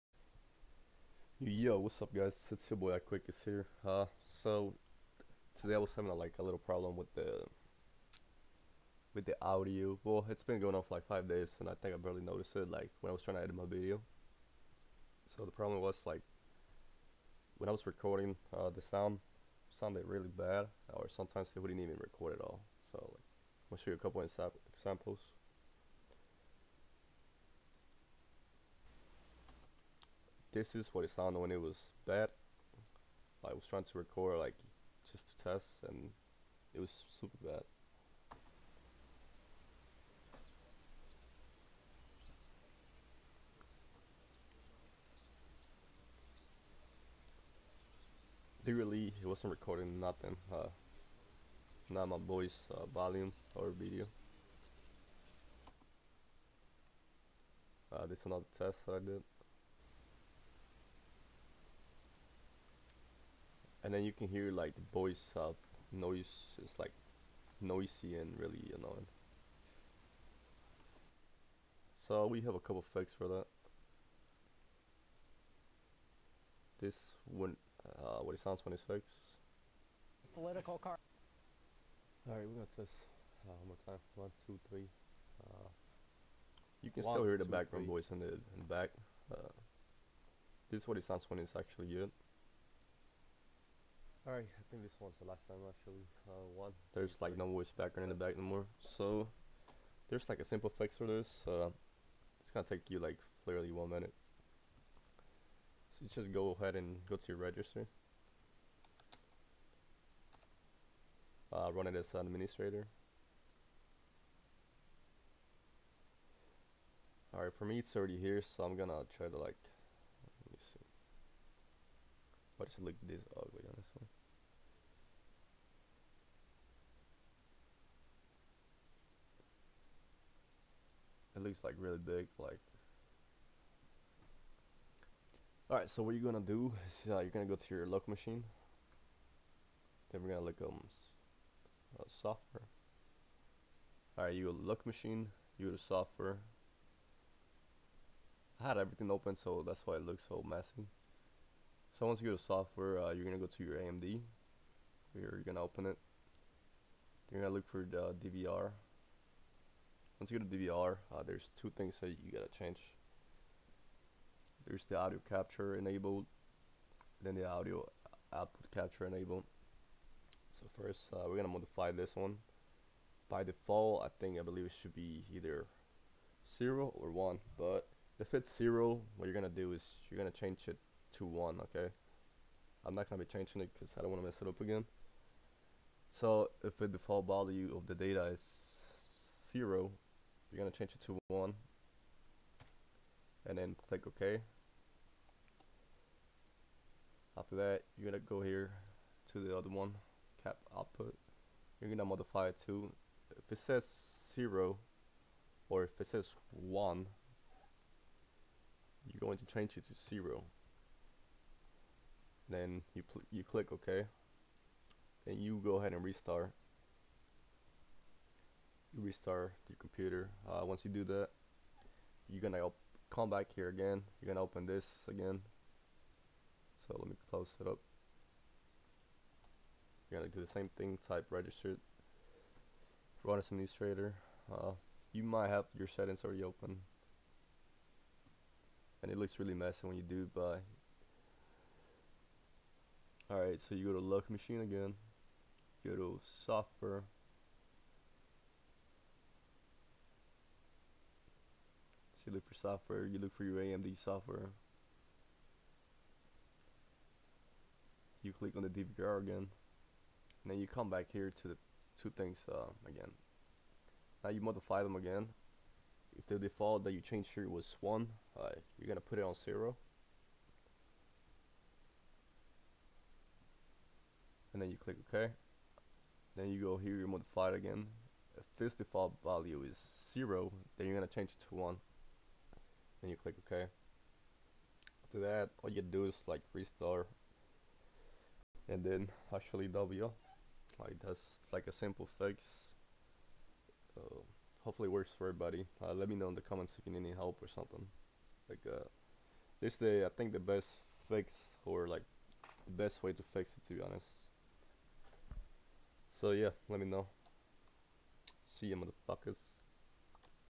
How to fix Amd Relive Not recording your audio or Gameplay!
this is a simple fix to a problem that happens when you update drivers